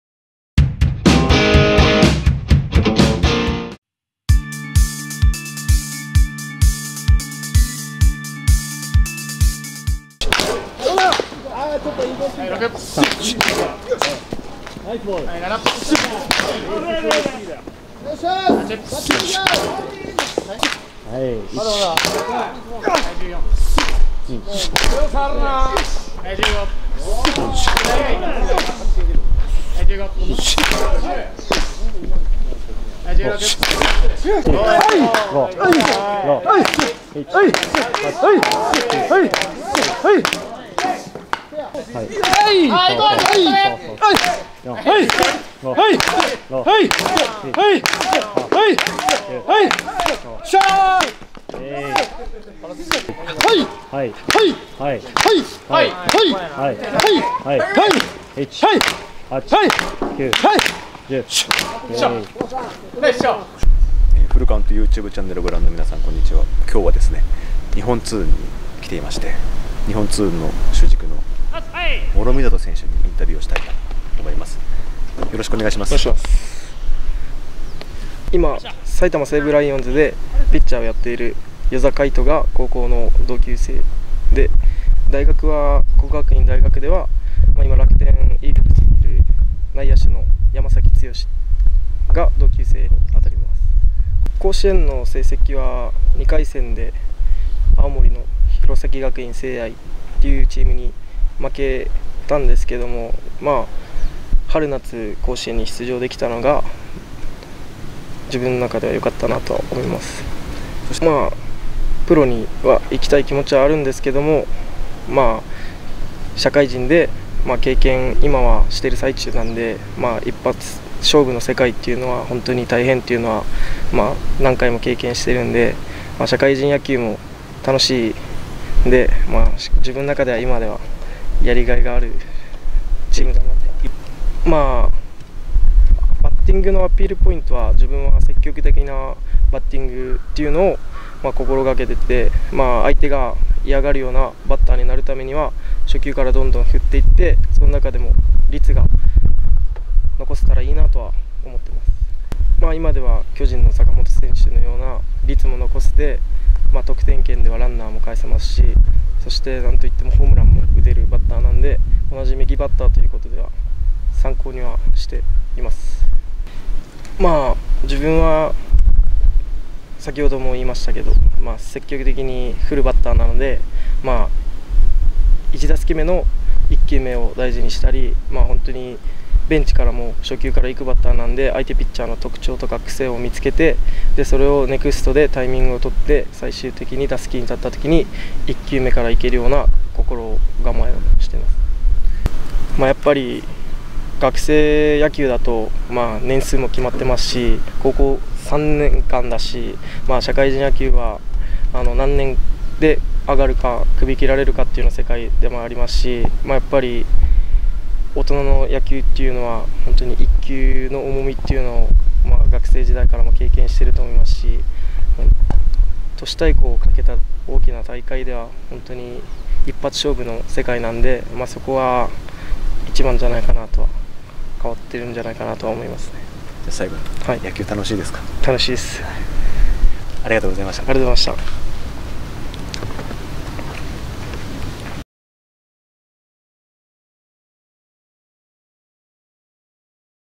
【今秋ドラフト候補】都市対抗野球出場の日本通運・諸見里匠内野手のマシンガンティー打撃練習 後半はインタビュー
7月13日から始まる第90回都市対抗野球大会が開幕する。5年連続44回目の出場となる強豪、日本通運の諸見里匠内野手は、国学院大4年だった2年前のドラフトで指名漏れを経験。悔しさをバネにプロの舞台を目指し、勝負の年に挑んでいる。

　沖縄県出身の24歳。沖縄尚学では1年から遊撃のレギュラーを掴み、3年時には主将として春夏ともに甲子園に出場した。進学した国学院大では1年の春からリーグ戦に出場し、高校に続き大学でも主将を務め、4年春にはベストナインを獲得した。

　笑顔が素敵な諸見里選手の真剣な打撃練習と、打撃へのこだわりをお聞きください！